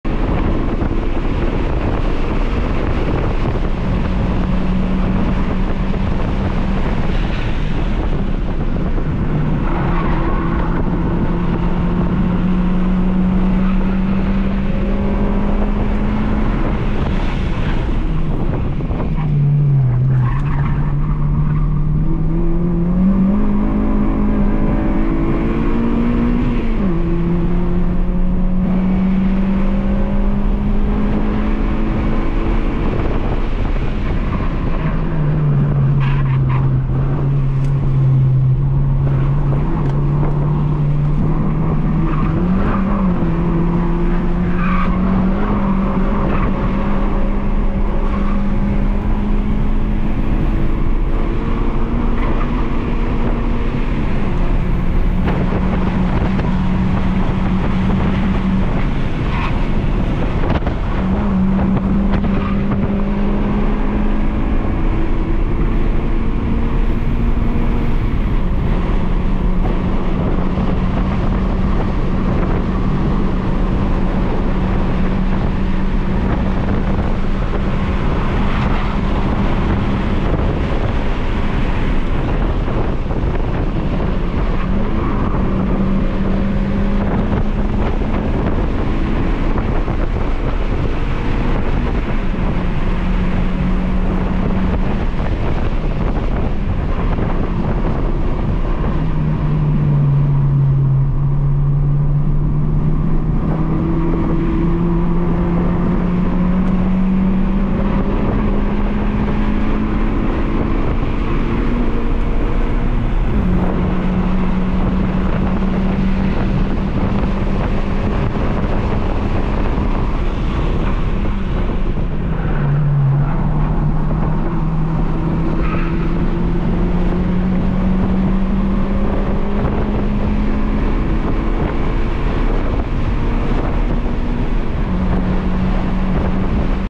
Honda Civic Si EP3 - Buttonwillow 13CW - 2:10.99
First time at Buttonwillow, with 1 hour on AC to learn the track beforehand. Bent a wheel after this lap :(

Speed Ventures 3/31/24
Stock K20A3, 5 speed, no LSD, full interior, with passenger
I/E, 136 WHP / 125 TQ
Hankook RS4
MeisterR GT1